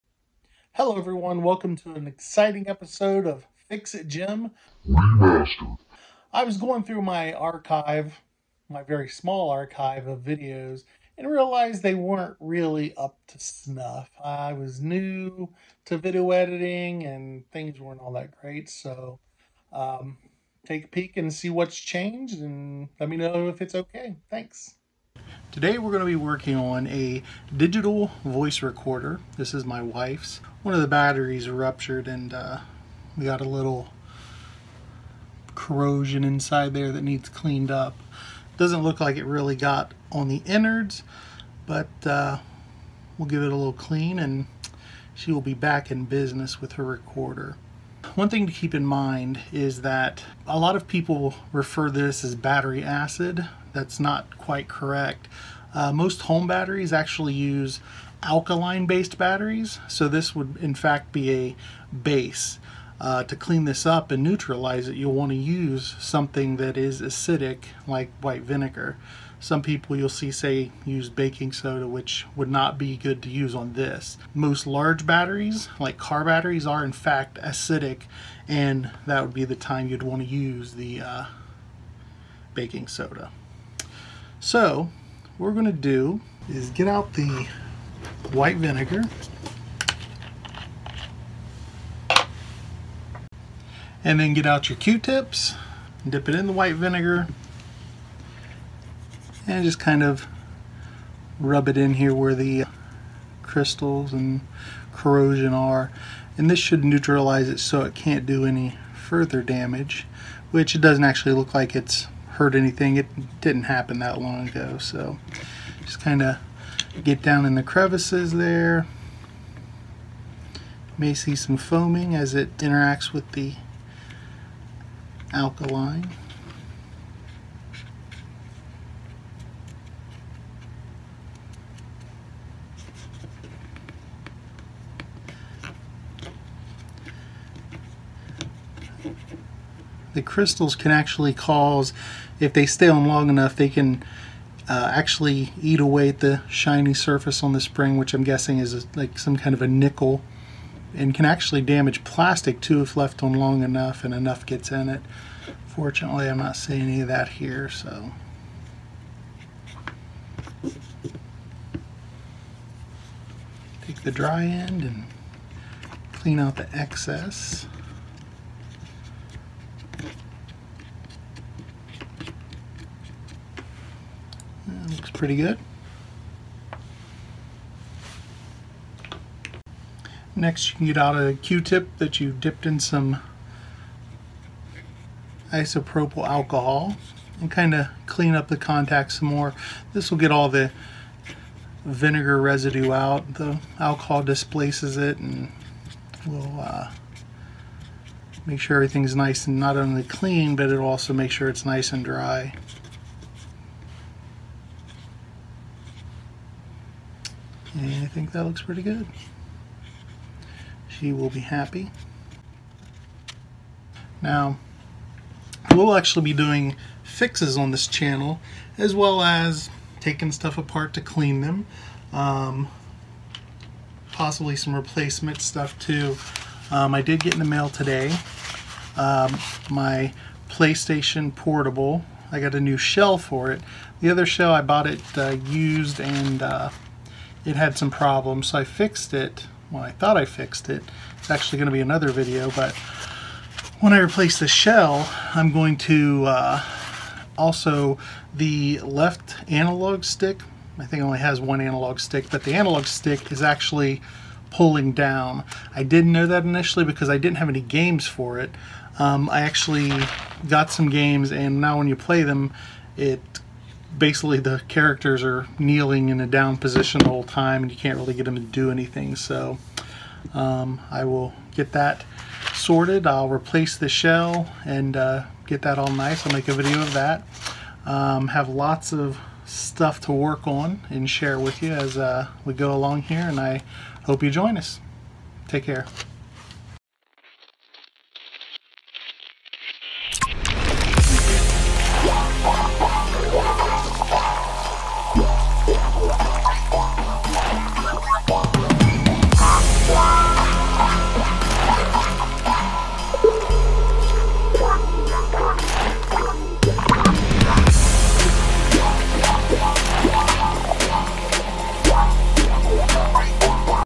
Cleaning Battery Corrosion from a Voice Recorder - REMASTERED
In this pilot episode, I clean up some battery corrosion in an Olympus WS-6005 Digital Voice Recorder.